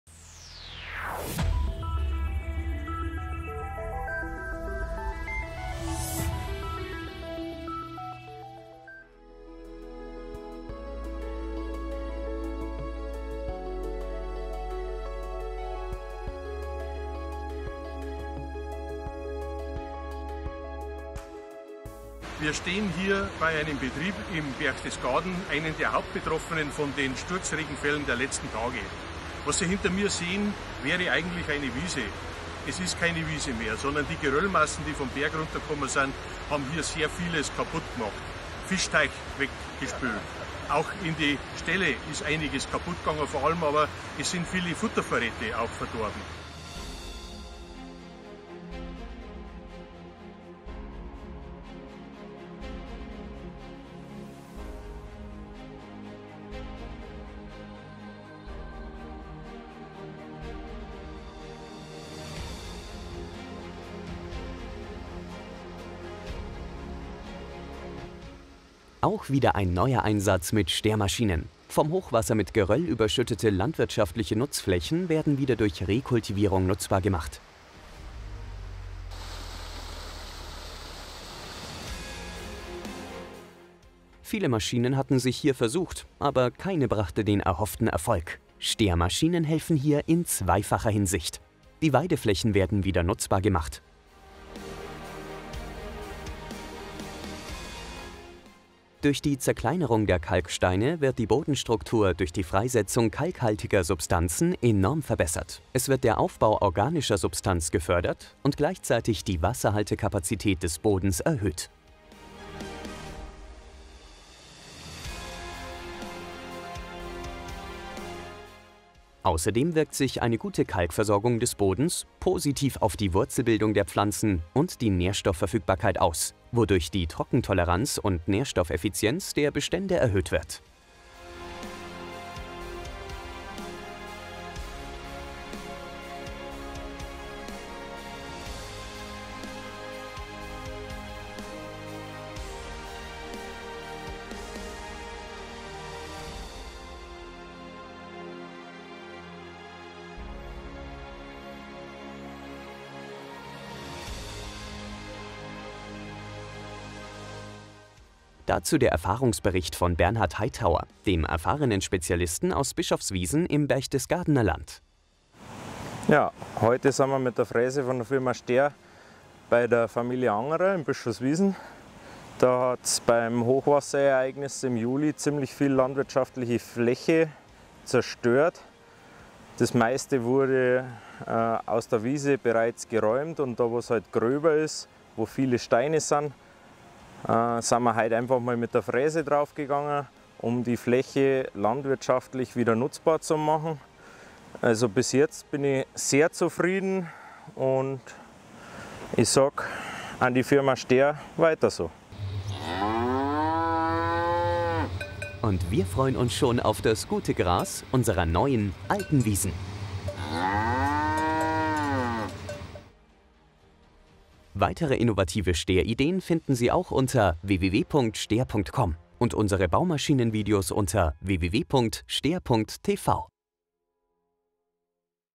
Rekultivierung Weideflächen mit Stehr Steinbrecher nach Hochwasser im Berchtesgadener Land
Rekultivierung von Weideflächen mit dem Stehr Steinbrecher SBB 240 die bei dem Hochwasser im Juli 2021 im Berchtesgadener Land völlig zerstört wurden.
Unter Rekultivierung werden technisch und materiell aufwendige Maßnahmen zur Wiederherstellung oder Rückführung einer Landschaft in einen nutzbaren Zustand verstanden.